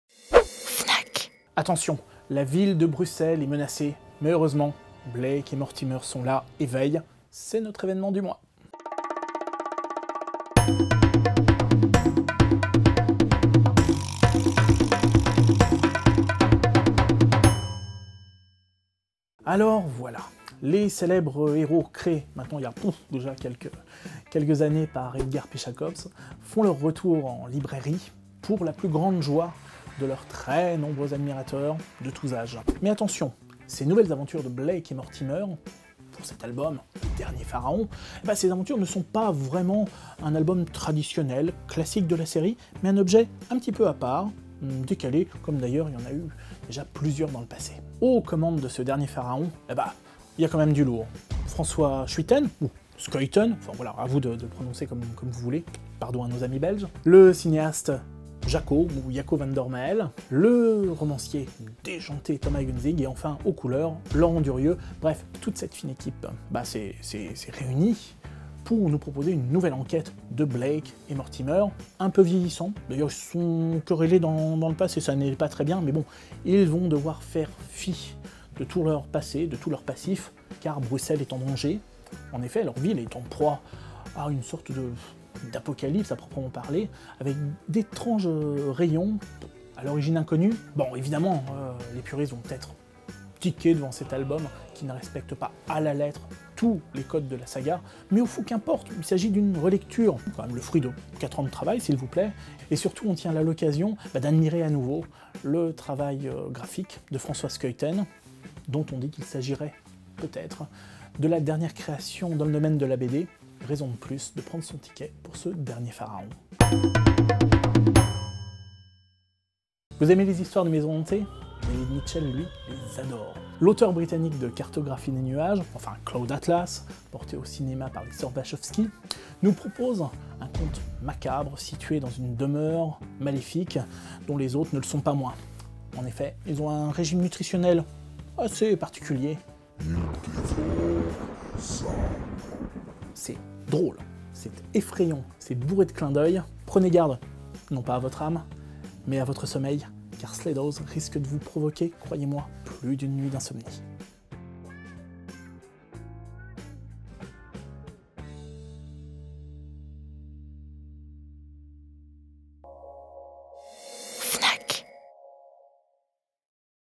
L'Instant Lire à la Fnac : Blake et Mortimer & David Mitchell
Ce mois-ci, Baptiste Liger, directeur de la rédaction du magazine Lire, nous propose de découvrir, ou redécouvrir la fabuleuse série de Blake et Mortimer. Pour ce nouvel opus, le casting est exceptionnel : François Schuiten, Jaco Van Dormael, Thomas Gunzig et Laurent Durieux.
Baptiste Liger nous parle également du nouveau roman du britannique David Mitchell, auteur du célèbre Cloud Atlas.